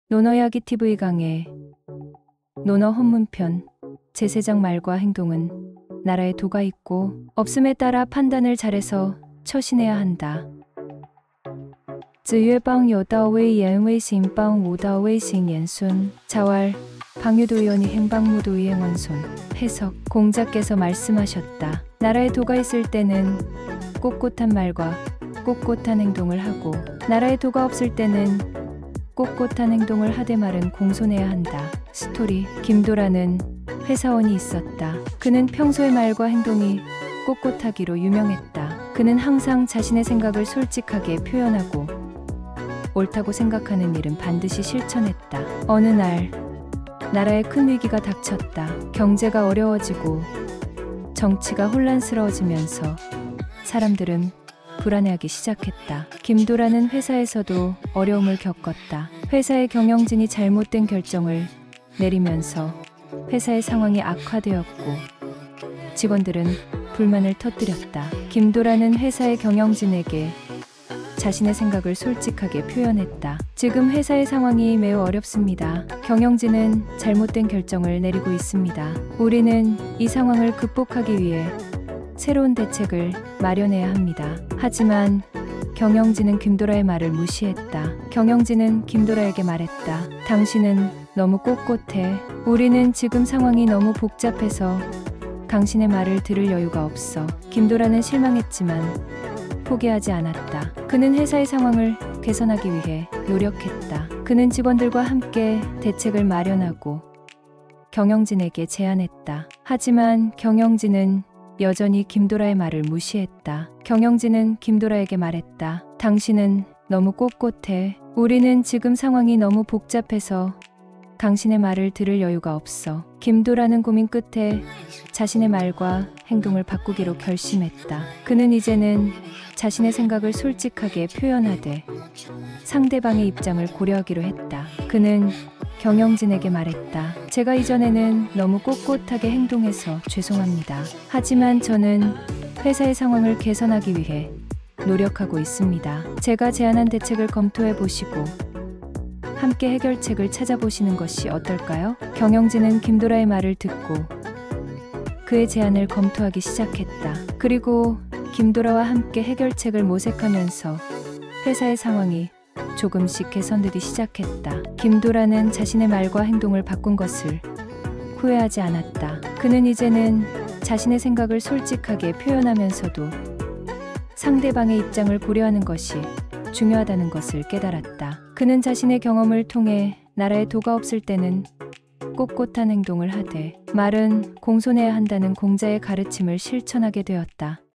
논어이야기 헌문편 제3장 말과 행동은 나라에 도가 있고 없음에 따라 판단을 잘해서 처신해야 한다
논어이야기TV강의, 논어 헌문편 제3장 말과 행동은 나라에 도가 있고 없음에 따라 판단을 잘해서 처신해야 한다

子曰：「邦有道，危言危行；邦無道，危行言孫。」

자왈: "방유도，위언위행；방무도，위행언손.“

신출한자

"孫(손자 손)“

해석

공자께서 말씀하셨다. "나라에 도가 있을 때는 꼿꼿한 말과 꼿꼿한 행동을 하고, 나라에 도가 없을 때는 꼿꼿한 행동을 하되 말은 공손해야 한다“

스토리

김도라는 회사원이 있었다. 그는 평소에 말과 행동이 꼿꼿하기로 유명했다. 그는 항상 자신의 생각을 솔직하게 표현하고, 옳다고 생각하는 일은 반드시 실천했다.

어느 날, 나라에 큰 위기가 닥쳤다. 경제가 어려워지고, 정치가 혼란스러워지면서 사람들은 불안해하기 시작했다.

김도라는 회사에서도 어려움을 겪었다. 회사의 경영진이 잘못된 결정을 내리면서 회사의 상황이 악화되었고, 직원들은 불만을 터뜨렸다.

김도라는 회사의 경영진에게 자신의 생각을 솔직하게 표현했다. "지금 회사의 상황이 매우 어렵습니다. 경영진은 잘못된 결정을 내리고 있습니다. 우리는 이 상황을 극복하기 위해 새로운 대책을 마련해야 합니다."

하지만 경영진은 김도라의 말을 무시했다. 경영진은 김도라에게 말했다. "당신은 너무 꼿꼿해. 우리는 지금 상황이 너무 복잡해서 당신의 말을 들을 여유가 없어."

김도라는 실망했지만, 포기하지 않았다. 그는 회사의 상황을 개선하기 위해 노력했다. 그는 직원들과 함께 대책을 마련하고, 경영진에게 제안했다.

하지만 경영진은 여전히 김도라의 말을 무시했다. 경영진은 김도라에게 말했다. "당신은 너무 꼿꼿해. 우리는 지금 상황이 너무 복잡해서 당신의 말을 들을 여유가 없어."

김도라는 고민 끝에 자신의 말과 행동을 바꾸기로 결심했다. 그는 이제는 자신의 생각을 솔직하게 표현하되, 상대방의 입장을 고려하기로 했다.

그는 경영진에게 말했다. "제가 이전에는 너무 꼿꼿하게 행동해서 죄송합니다. 하지만 저는 회사의 상황을 개선하기 위해 노력하고 있습니다. 제가 제안한 대책을 검토해 보시고, 함께 해결책을 찾아보시는 것이 어떨까요?"

경영진은 김도라의 말을 듣고, 그의 제안을 검토하기 시작했다. 그리고 김도라와 함께 해결책을 모색하면서 회사의 상황이 조금씩 개선되기 시작했다.

김도라는 자신의 말과 행동을 바꾼 것을 후회하지 않았다. 그는 이제는 자신의 생각을 솔직하게 표현하면서도 상대방의 입장을 고려하는 것이 중요하다는 것을 깨달았다.

그는 자신의 경험을 통해 나라에 도가 없을 때는 꼿꼿한 행동을 하되 말은 공손해야 한다는 공자의 가르침을 실천하게 되었다.

*위의 영상은 Vrew에서 생성하였습니다.